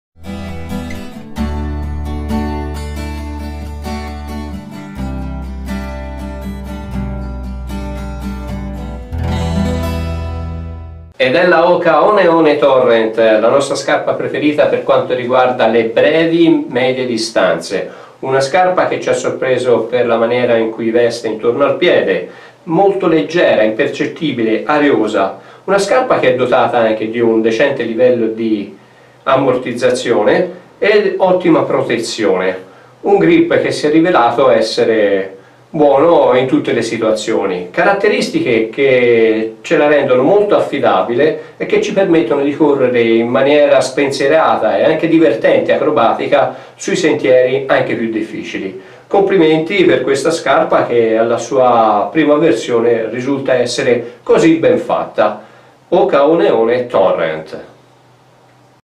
Hoka One One Torrent
Recensione del modello trail running Hoka One One da allenamento e gare su brevi e medie distanze